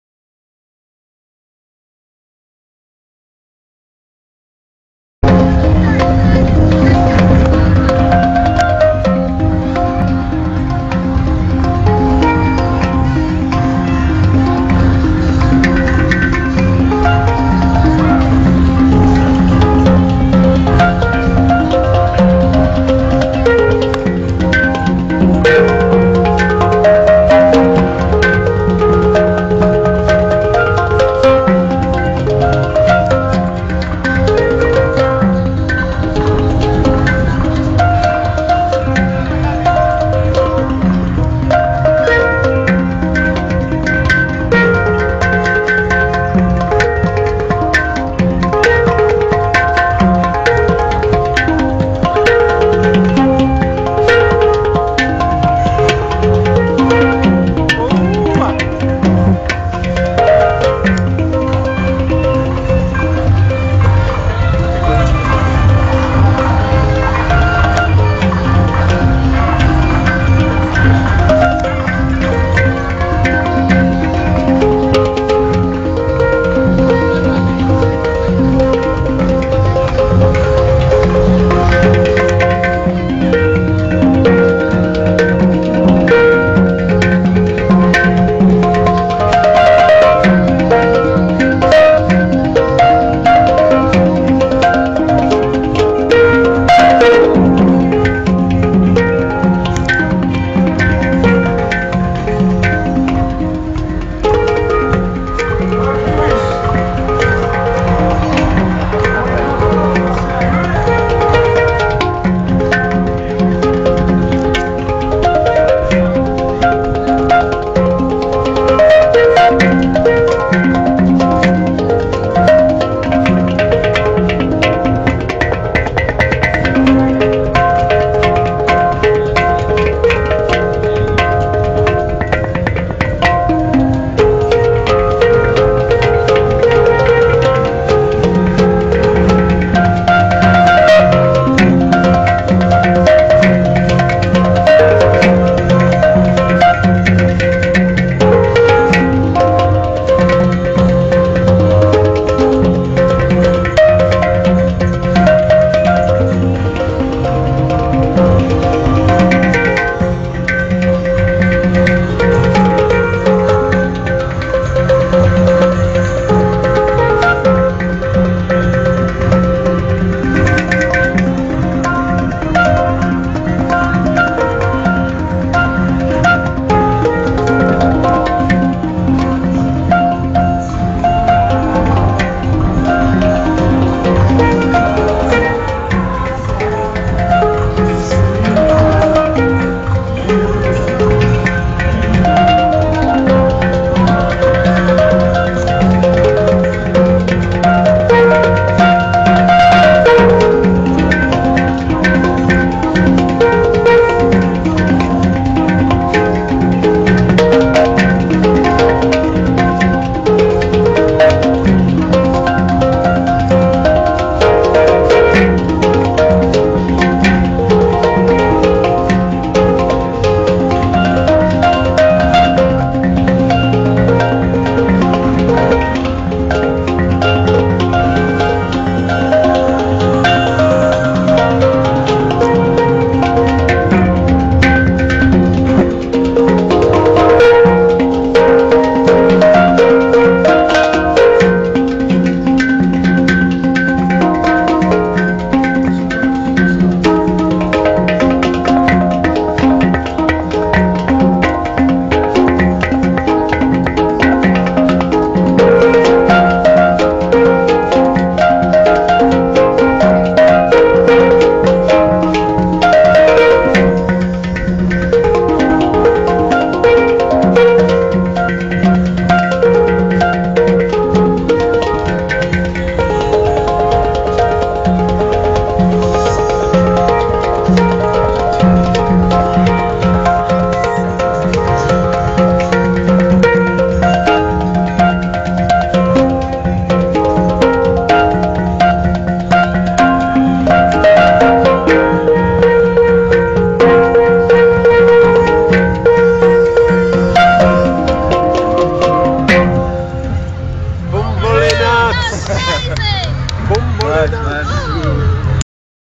in2nation & davide swarup
this vid was taken after we played on stage at the love box festival !!  sound is not so good  but hay  we got the best plays here playing together !!  so we had to record !!! :-)))  thanks guys